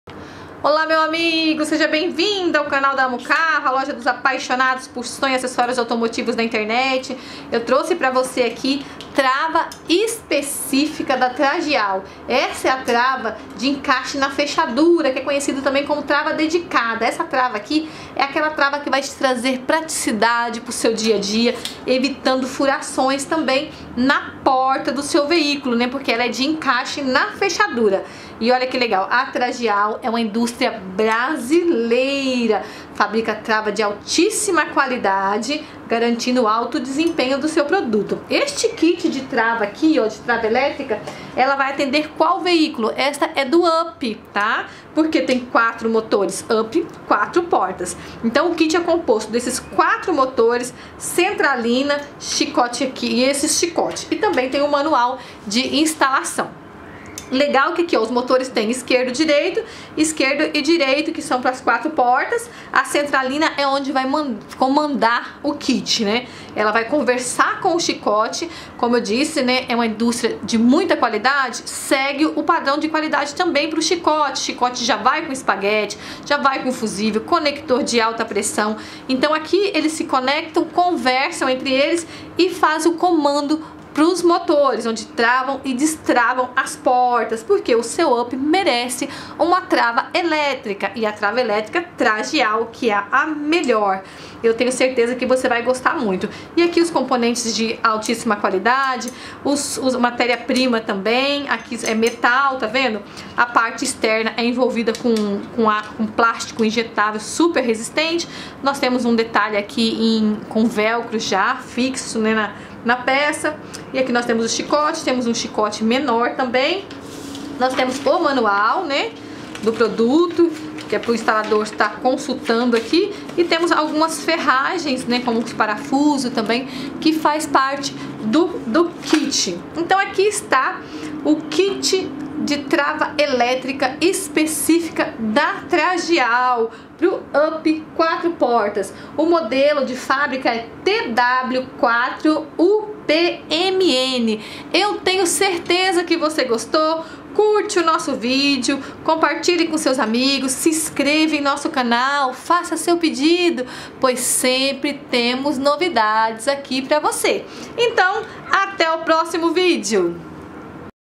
Kit Trava Elétrica Up 4 Portas Mono Tragial TW4UP Original 4P
Trava elétrica da Tragial! Vem conhecer e garantir a sua. Resistente, Eficiente, de alta qualidade para garantir a sua segurança e a do seu veículo. Confira algumas características:
4 Portas
Material de Qualidade

O Kit Trava Elétrica Up 4 Portas Mono Tragial TW4UP é boa e se aplica para quais modelos?
Sim! O Kit Trava Elétrica Up 4 Portas Mono Tragial TW4UP é boa e se aplica para o Up 4 Portas.